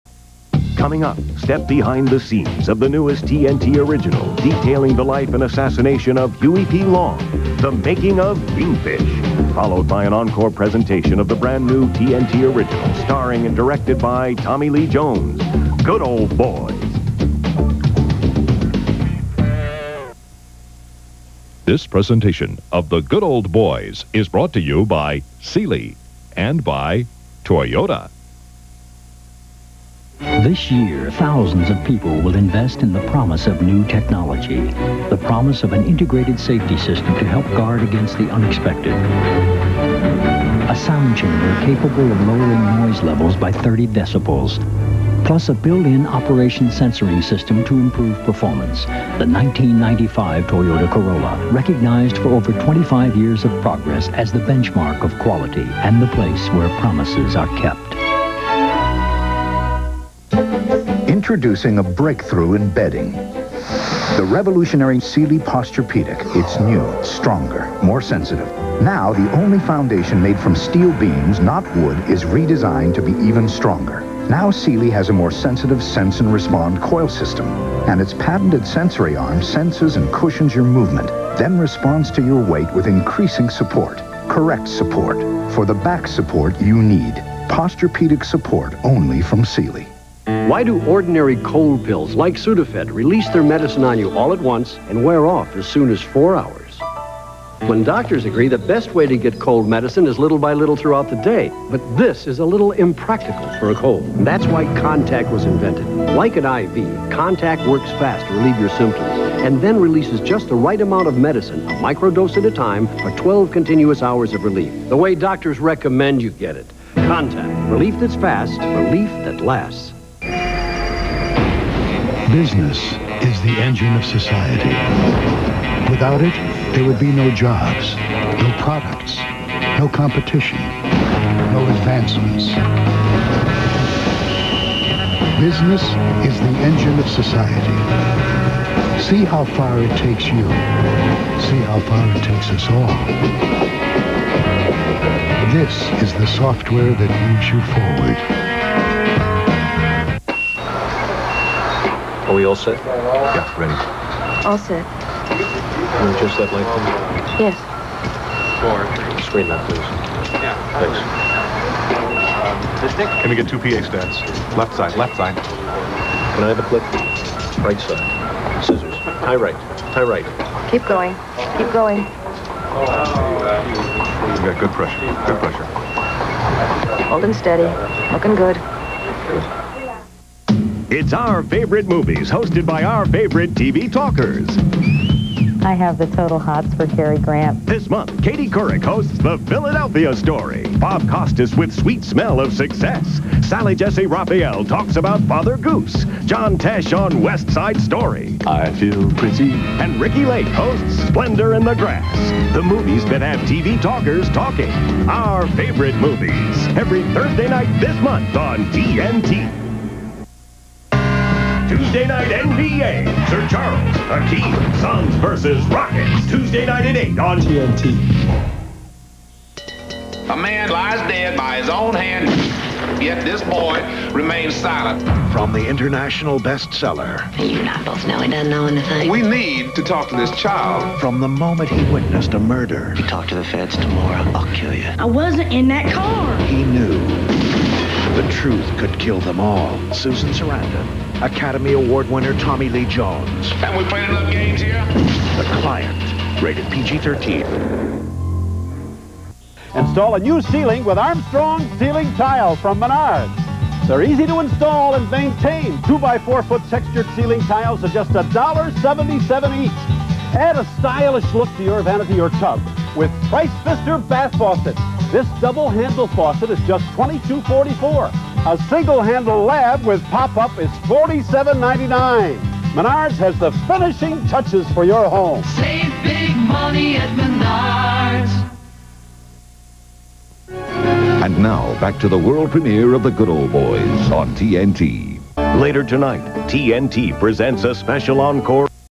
TNT - March 5, 1995 Commercials
Found: Craigslist
Description: During Good Old Boys. TNT; March 5, 1995.